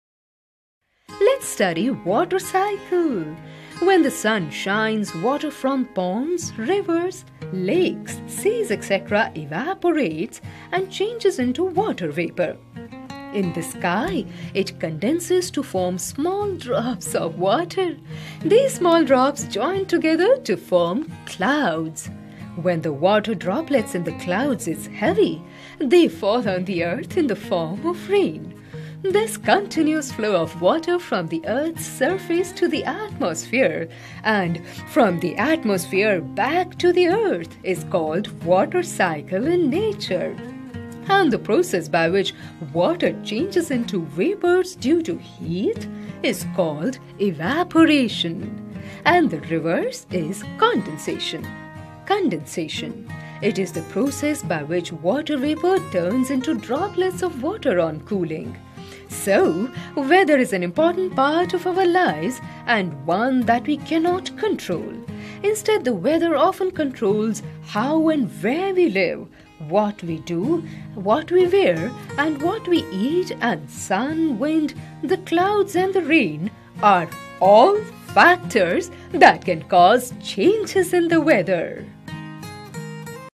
Science - Water Cycle - CBSE Class 4 Science (IV)
The Video can be used by students of CBSE, ISCE, NCERT and other Boards looking for concrete solutions in education.. Learn about water cycle - Evaporation and condensation process